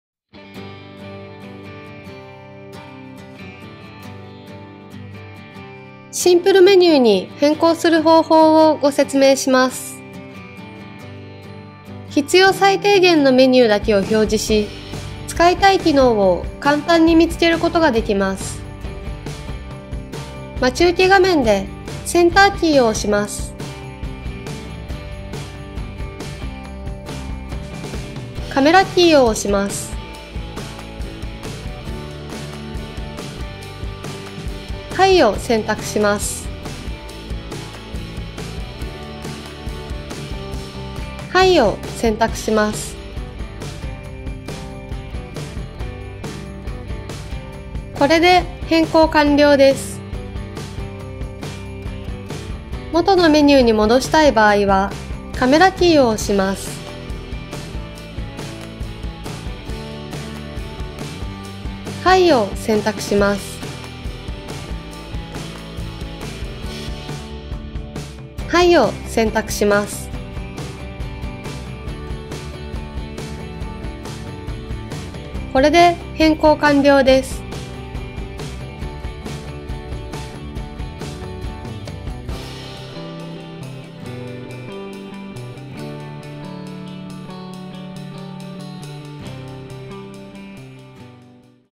【AQUOS K SHF33】シンプルメニューに変更する
シンプルメニューに変更する方法をご説明します。